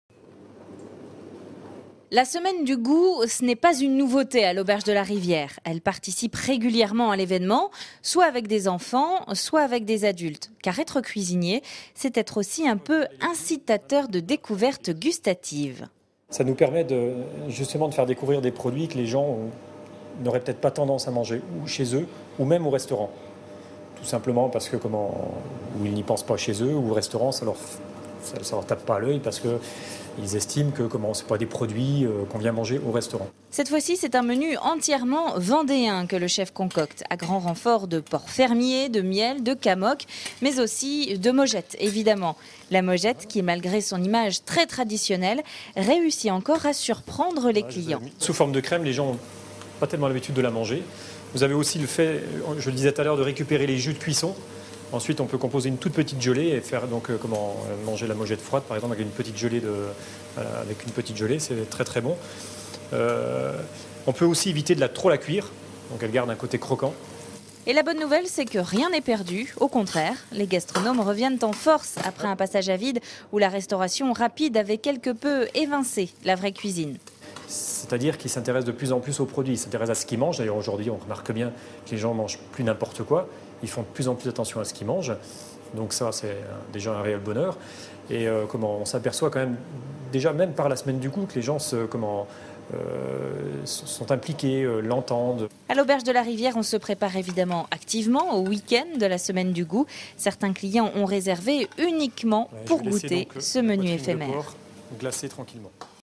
Semaine du goût : la Vendée au menu
Les chefs vendéens proposent de découvrir pour les uns et redécouvrir pour les autres, le patrimoine culinaire de la Vendée. (DROITS RESERVES . Pour toute exploitation sur TV, web, mobile, aller